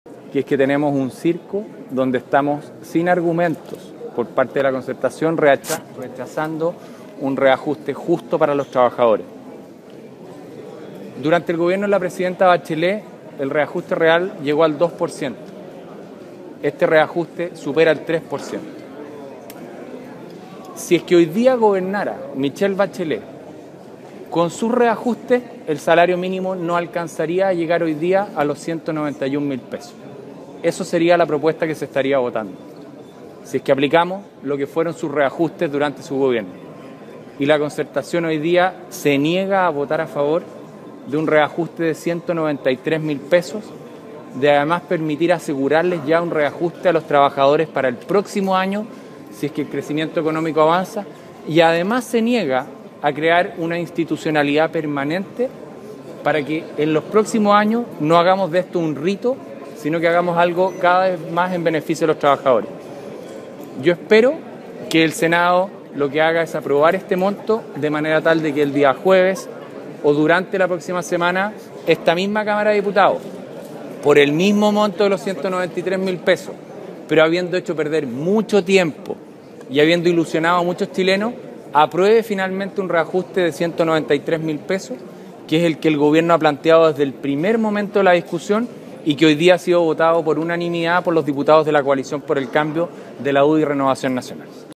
Silva: "Si hoy gobernara Bachelet, el salario mínimo no llegaría a los $191 mil"
El presidente de la Comisión de Hacienda lamentó el rechazo de los parlamentarios de la Concertación al proyecto sobre ingreso mínimo, acusando que en términos reales significaba un aumento superior a los que hubo durante la administración pasada.

Tras una estrecha votación, la Cámara de Diputados rechazó este martes el proyecto que reajusta el salario mínimo, aumentando esa cifra desde $182 mil a $193 mil, propuesta hecha por el Gobierno y respaldada unánimemente por los diputados de la UDI.

Para el presidente de la Comisión de Hacienda de la Cámara Baja, Ernesto Silva (UDI), "esta es una noticia lamentable, ante una propuesta que significaba una mejora para el país".

Silva criticó con dureza a sus pares de la oposición, evidenciando que durante el gobierno de Michelle Bachelet el reajuste del salario mínimo, en términos reales, fue de 2%, mientras que la propuesta de la administración Piñera llegaba al 3% real.

"Durante el gobierno de la presidenta Bachelet, el reajuste real llegó a 2%; este reajuste supera el 3%", dijo el parlamentario gremialista, enfatizando luego que "si hoy día gobernara Michelle Bachelet, con sus reajustes, el salario mínimo no alcanzaría a llegar a los $191 mil".

El legislador lamentó asimismo que la iniciativa gubernamental "permitía no sólo subir el salario mínimo en un 6% (nominal), sino que además resolvía el reajuste del próximo año para que los chilenos no tuvieran que vivir lo que está ocurriendo hoy, que tenemos un circo en que la Concertación, sin argumentos, rechaza un reajuste justo para los trabajadores". Esto, porque el Ejecutivo busca establecer una institucionalidad permanente para enfrentar esta discusión.

"Espero que el Senado apruebe este monto, para que el día jueves o la próxima semana, esta misma Cámara de Diputados, por el mismo monto y después de haber perdido mucho tiempo y habiendo ilusionado a muchos chilenos, apruebe finalmente un reajuste de $193 mil que es el que Gobierno ha propuesto y que los diputados de la Coalición por el Cambios hemos votado unánimemente a favor", finalizó.